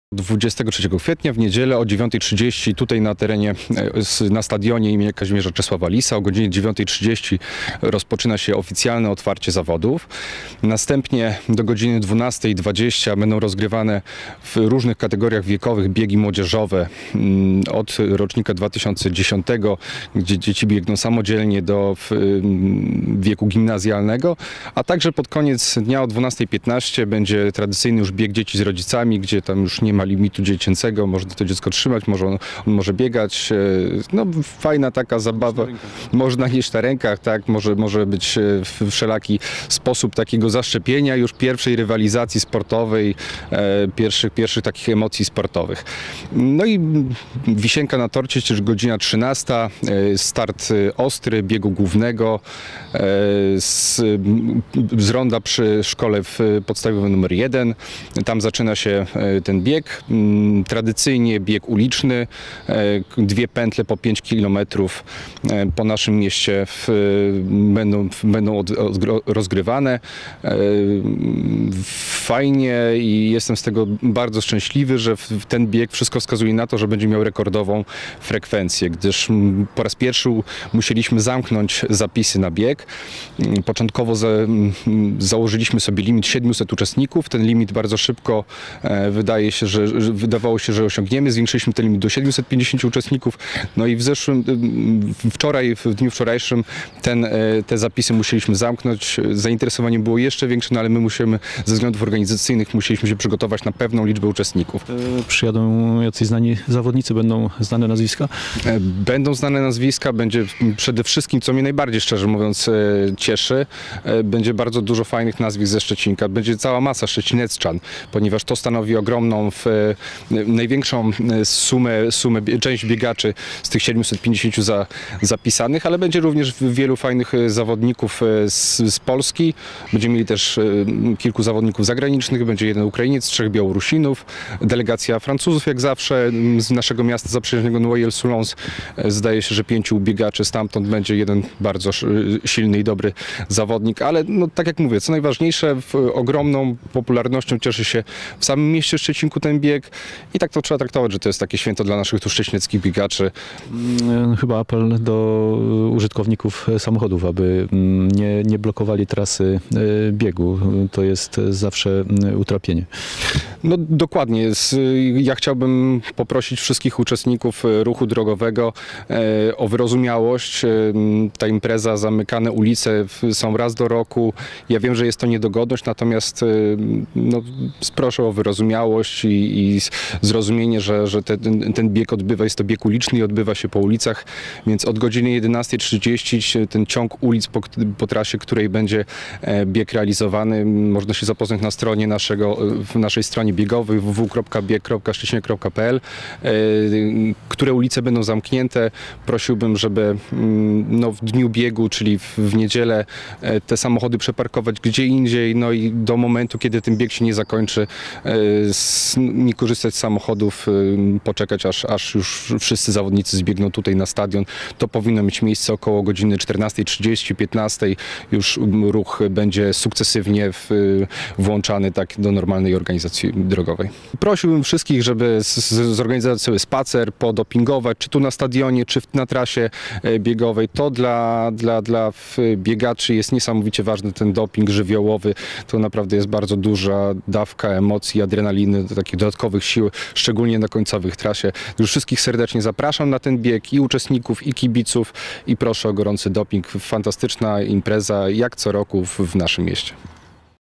Uliczne bieganie [2017.04.21]
Już po raz trzydziesty czwarty ulicami Szczecinka pobiegną uczestnicy Międzynarodowego Biegu Ulicznego - Memoriału Winanda Osińskiego. Uroczyste otwarcie imprezy o godz. 930, a zaraz potem rozpoczną się biegi młodzieżowe na różnych, odpowiednich do wieku uczestników dystansach. O 12.30 rozpocznie się bieg dzieci z rodzicami, a start do biegu głównego zaplanowano na godzinę 13. Zakończenie około godziny 15.